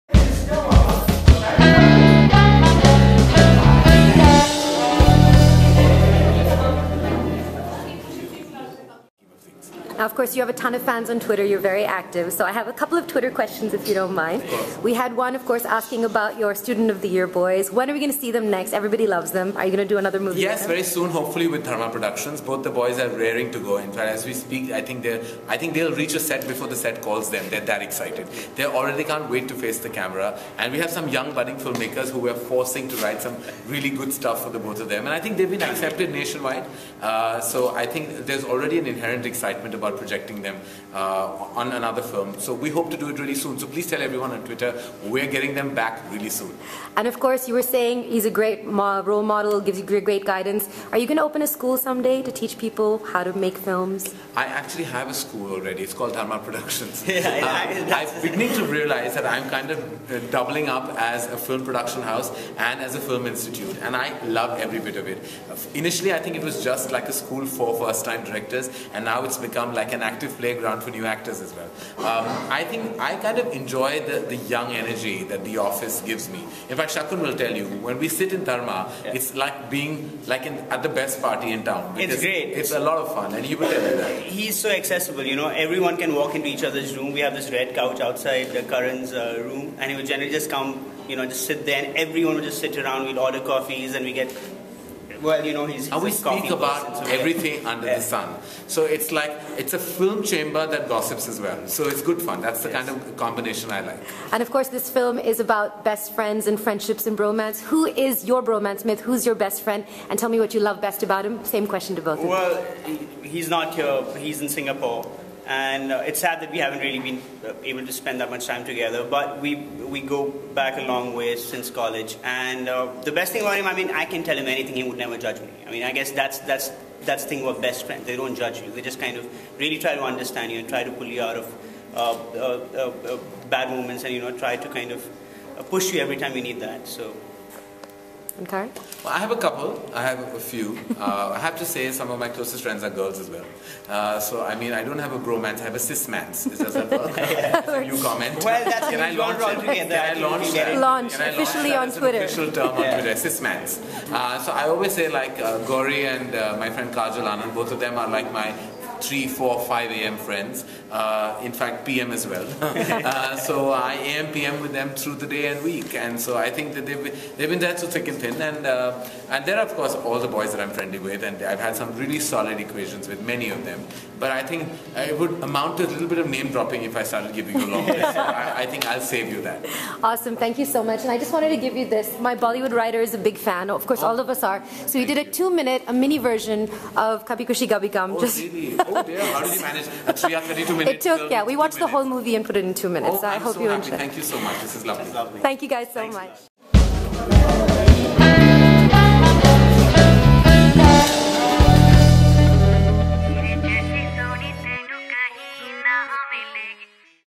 Karan Johar & Shakun Batra Answer Your Twitter Questions!
Kjo answers YOUR Twitter questions LIVE! Check it out :)